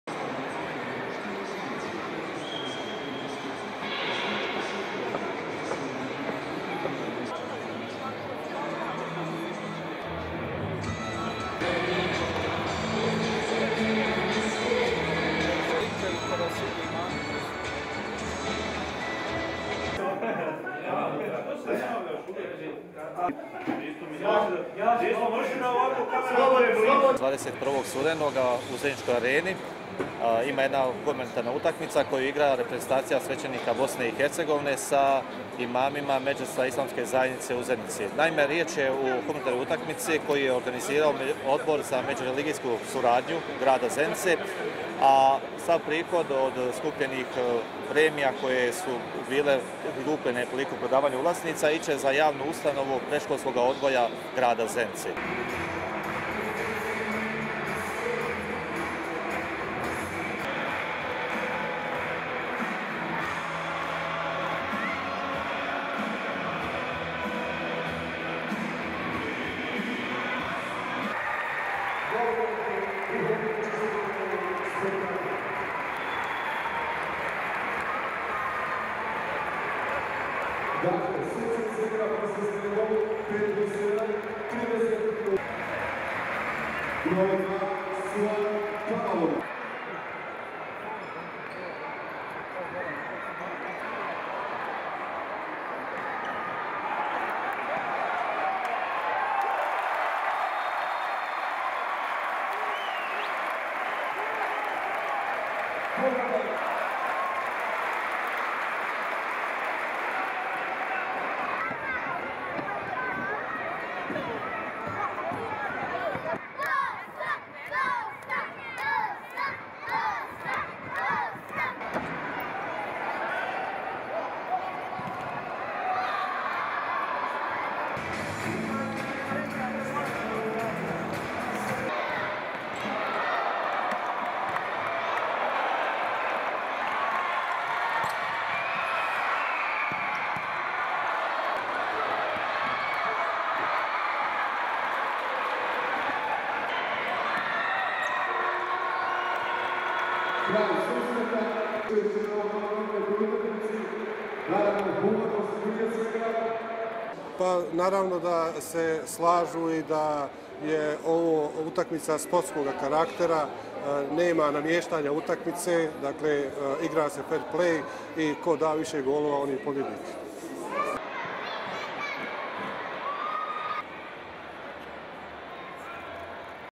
Catholic priests and imams play for pride in interfaith football match
1. Wide of Catholic priests and Muslim imams shaking hands on indoor football court in sports hall, dressed in religious clothing
2. Wide of imams standing in circle in middle of football court
3. Mid of priests and imam's standing in line to each other in the middle of court
4. Close up of Catholic priest standing between two Imams
5. Various of priests taking off religious clothes and putting on football shirts in dressing room
6. SOUNDBITE (Croatian): Father Zdravko Andjic, Franciscan Catholic priest from Zenica:
"21st of November in Zenica Arena, we are playing a game between Catholic priests and Muslim imams from Zenica. We want to play this humanitarian game, organised by the council of inter-religious cooperation in Zenica, and all income from sold tickets we will go to the new kindergarten in Zenica."
7. Various children from kindergarten that will receive proceeds from game dancing and singing in sports hall
8. Low shot of priests and imams walking onto court holding  hands with children
9. Wide of players waving to crowd, priests in white shirts, imams in green shirts
10. Wide of game being played
11. Wide of member of imam team scoring goal, crowd cheering, imam high-fiving teammate
12. Mid of game, priests in possesion (of football)
13. Wide of game from behind the goal, children watching the game in foreground
14. Mid of children in stands cheering (Bosnian) "Bosnia, Bosnia"
15. Mid of game, priests in possesion
16. Mid of imams during break
17. Wide of game, priest takes shot, misses
18. Mid of imams attacking priest's goal, goalkeeper saves
19. Wide of game, fans in stands in foreground
20. Wide of game, siren sounds end of the game
21. Mid of priests and imams shaking hands
22. SOUNDBITE (Bosnian): Jakub Salkica, Imam of Islamic community of Zenica:
"Of course we get along, this is a game, sports. There is no match fixing..We are playing fair-play, so who-ever scores more goals, he is the winner."
23. Mid of digital scoreboard showing result:  Imams 1 - Priests 3
24. Wide of priests, imams and referees lined up for team photo together
STORYLINE
Four thousand Bosnian football fans turned out for a game with a difference on Wednesday as Catholic priests and Muslim imams took each other on for charity.
In one of the most fragmented societies in Europe, the inter-religious committee organised a humanitarian football game of Bosnian Franciscans and Bosnian imams to collect the money for the re-building of Zenica's kindergarten.
Local authorities in Bosnia's fourth largest city were struggling to come up with funds so religious leaders of Bosnian churches decided that football was the answer.
The two teams have been playing against each other for three years but this time the sports hall was full.
At a price of two Bosnian marks (1.3 US dollars), the teams managed to gather enough to kick-start the kindergarten project.
Crowds cheered as the priests - in white - scored their third goal for a 3 -1 victory over the imams.
Hundreds of children, who will benefit from this project, cheered in the stands.
In a country swamped by political and national conflicts, it is rare to see an example of how religious communities can work together for mutual benefit.
"Of course we get along, this is a game," said Jakub Salkica, Imam of Islamic community of Zenica, smiling as he joked that the game wasn't fixed.